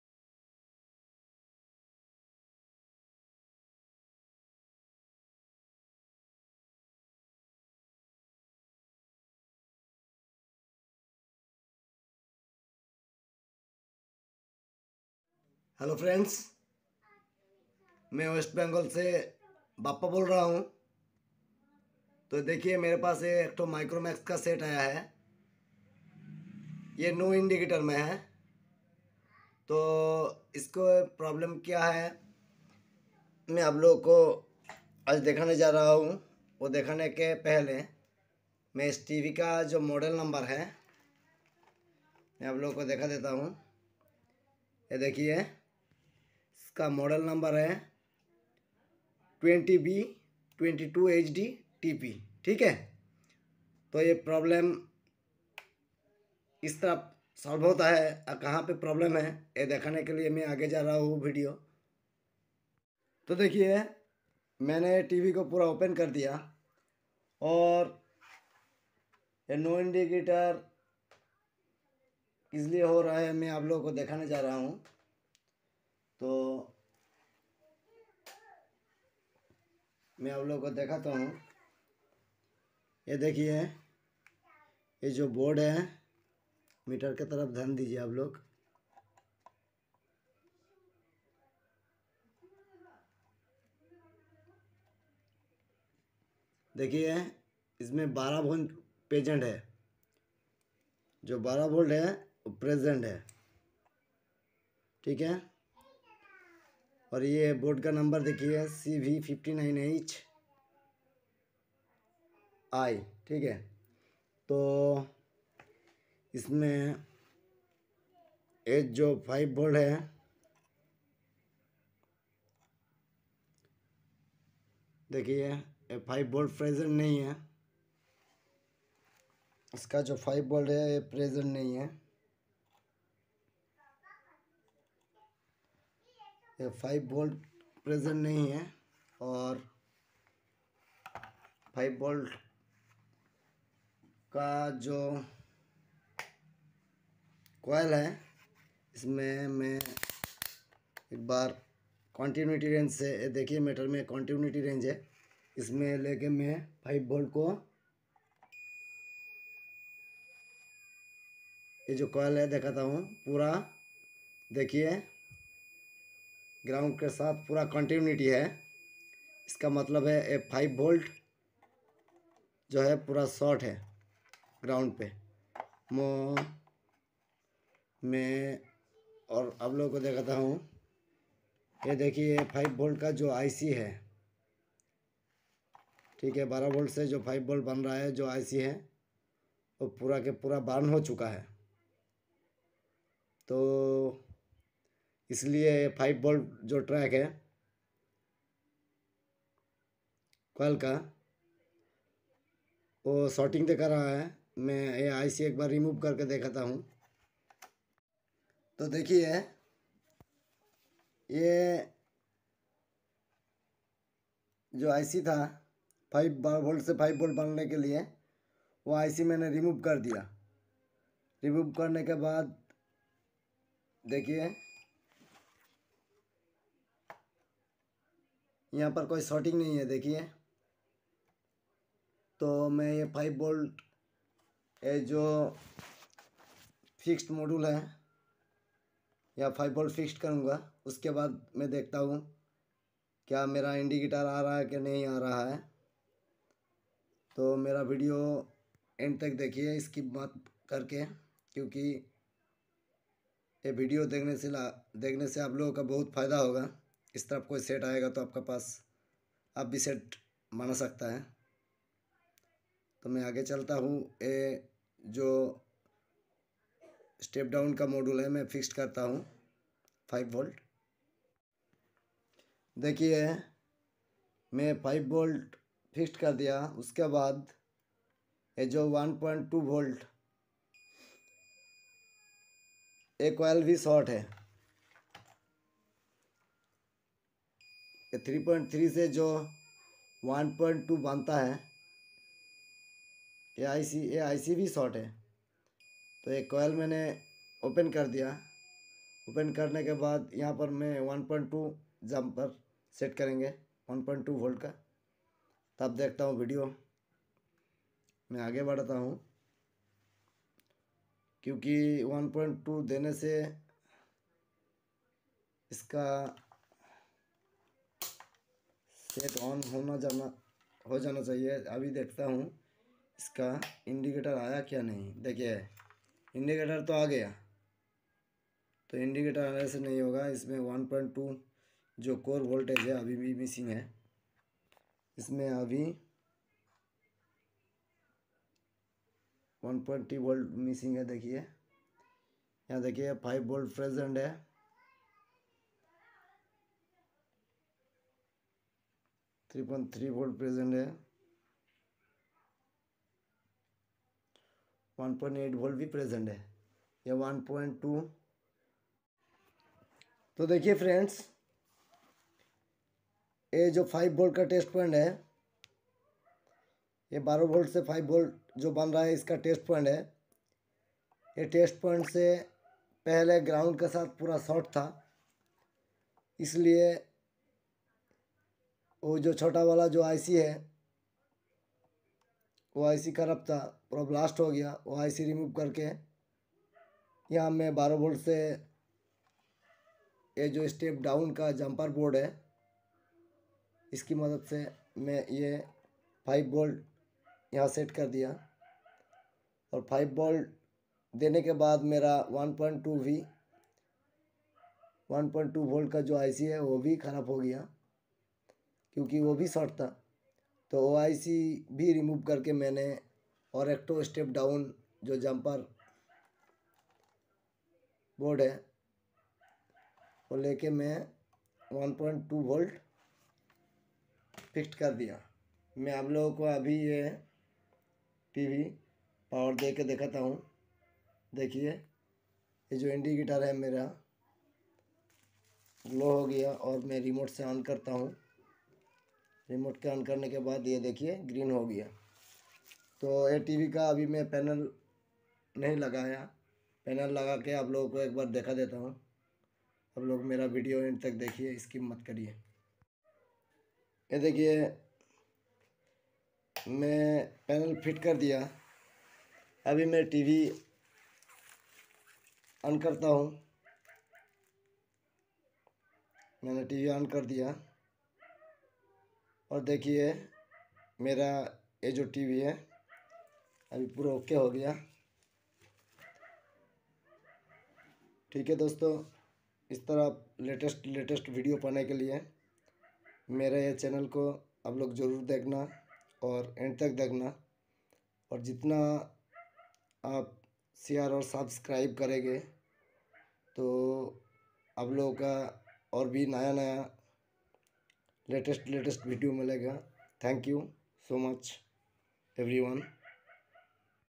MICROMAX 20B22HD NO INDIGATOR PROBLEM
no indigator problem. Micromax led set dead condition.power supply ok but set no indigator